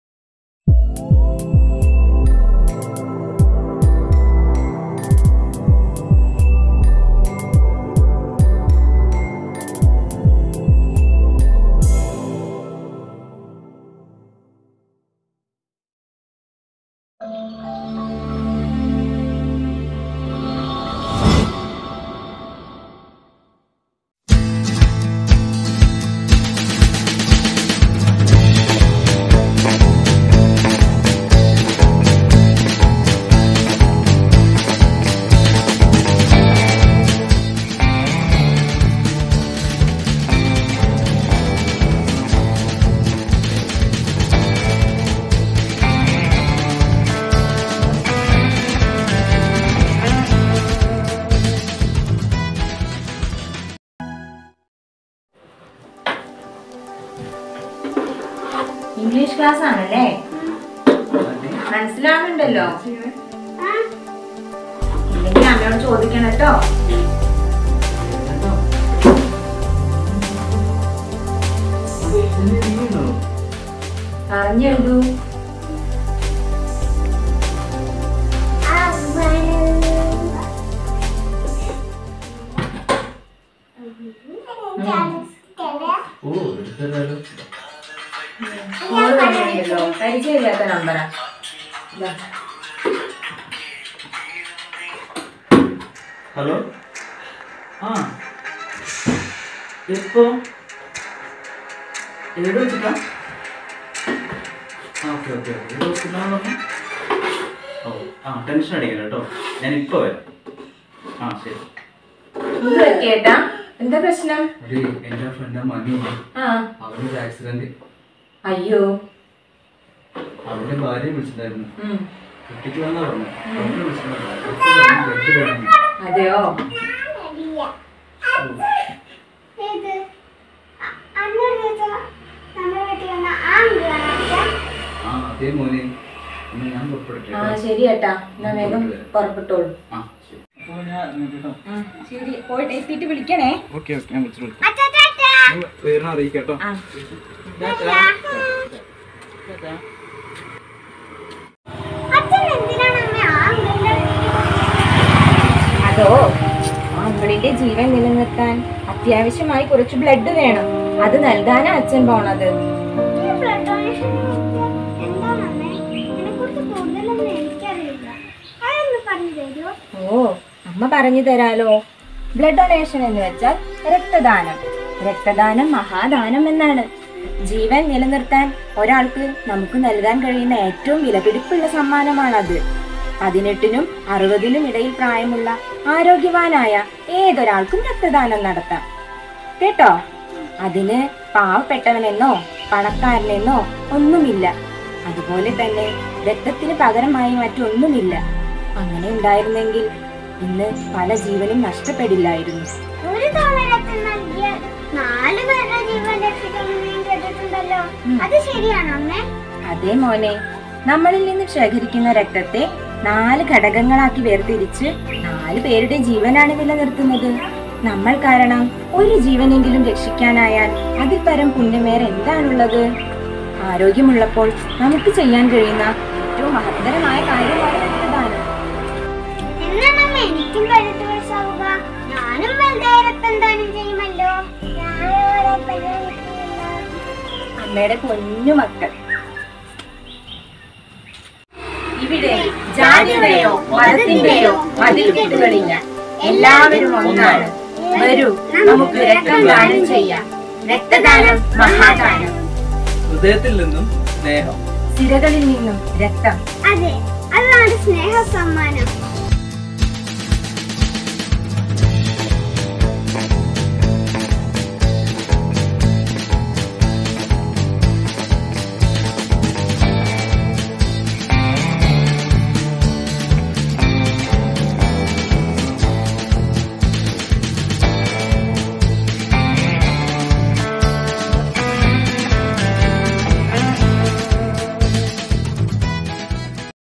Sneha Sammanam|സ്നേഹ സമ്മാനം | Blood Donation Day
Blood Donation Day | Sree | Kovilingal | Sneha Sammanam l The most precious gift in the world is blood donation.  We too can share in giving that precious gift ...